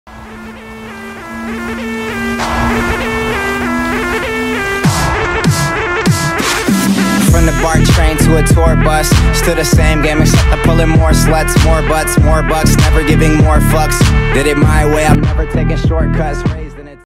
NSX Honda SENNA Edition | Short Edit
Yo

A short edit i have been working on.
CC looks dark after uploading idk why
Clips from THE-LOWDOWN.com (YT)